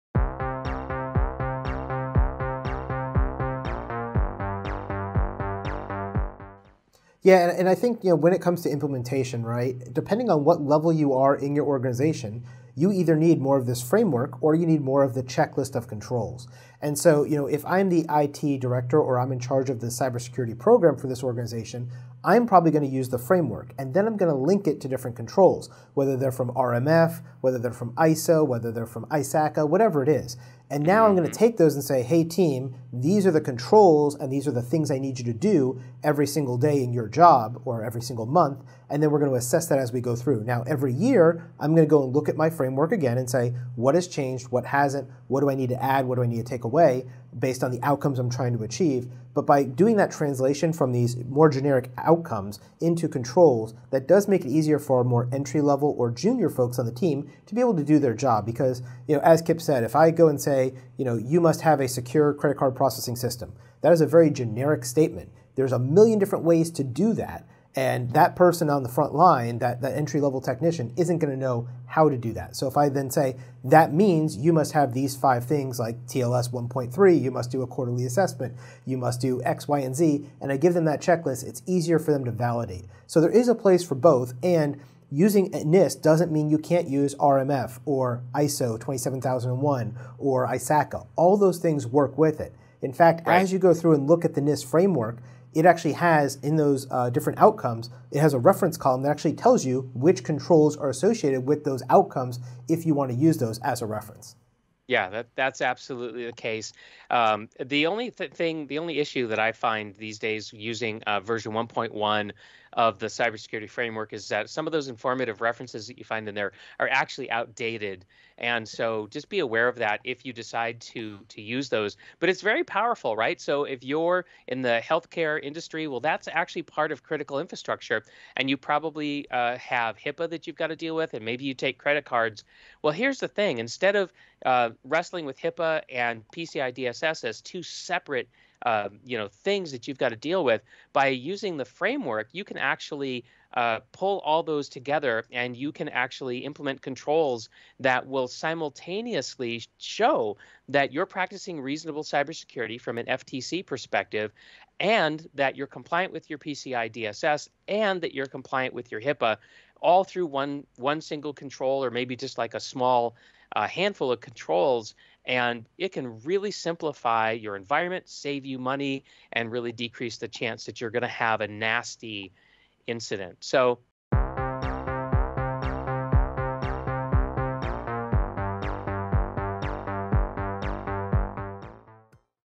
How to integrate other control methods into the NIST Cybersecurity Framework (Clip from Episode 62)
In this video we will show you how to integrate other control methods (ISACA, ISO, RMF) into the NIST Cybersecurity Framework.

for great podcast episodes!